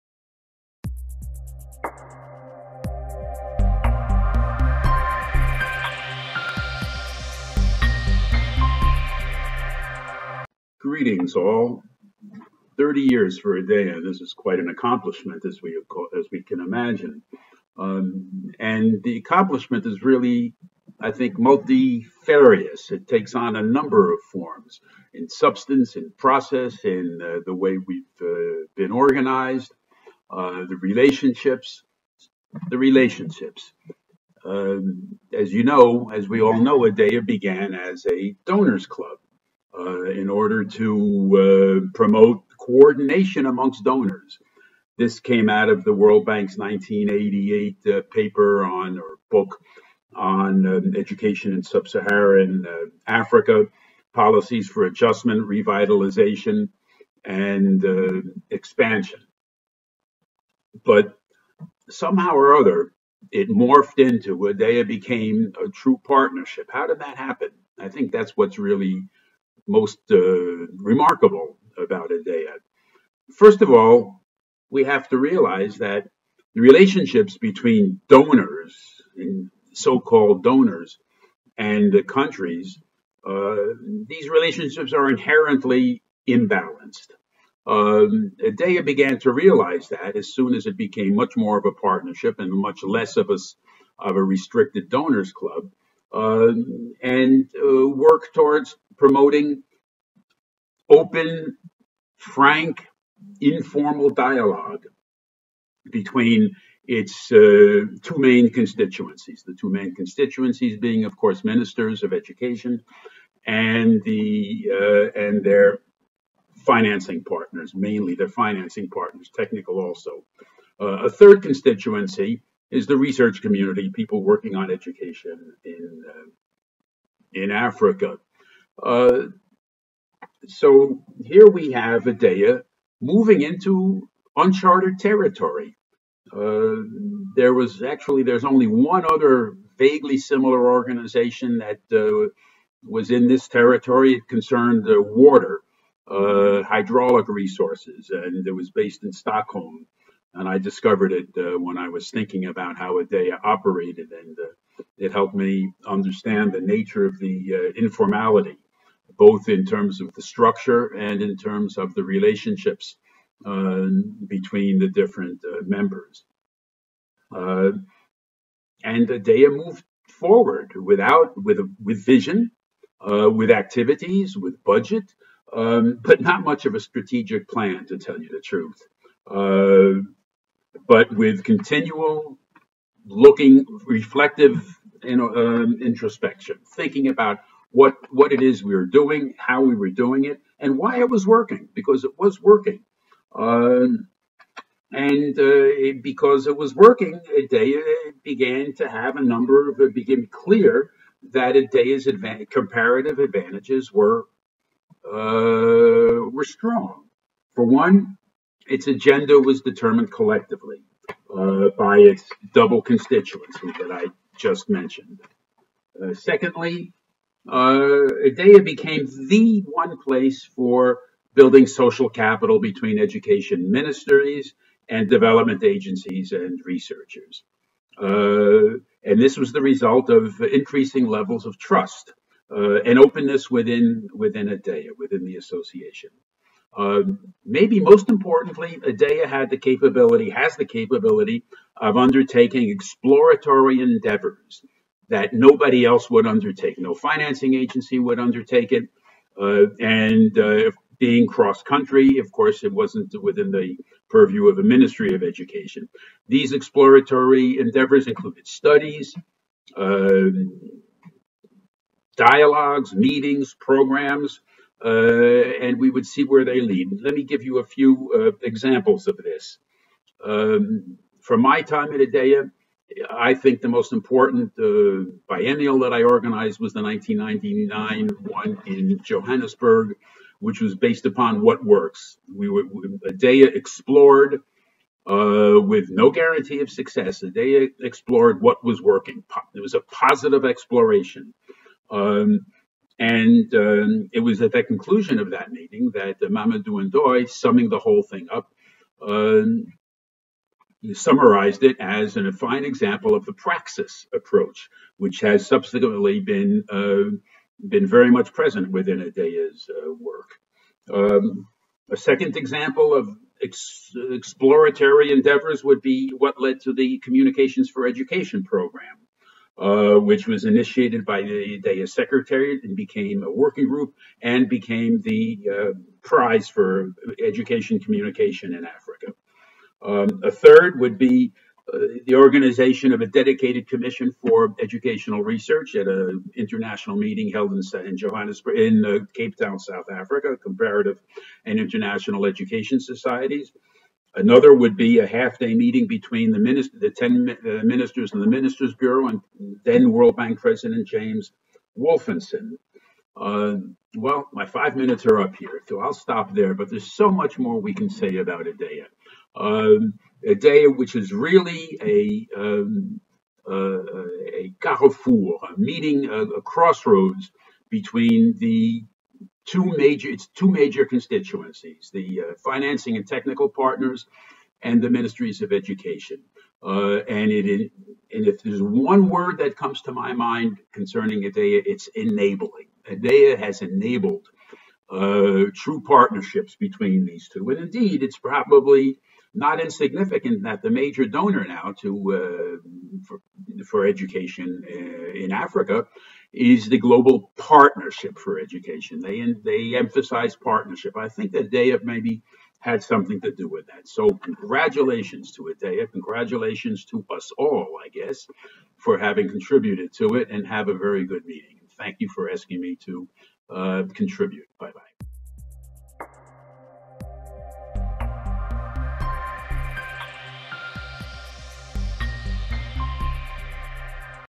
ADEA30: Testimonial Video - Richard Sack
ADEA30: Do you know? ADEA has been contributing for 30 years to education and training in Africa (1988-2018)...what a milestone!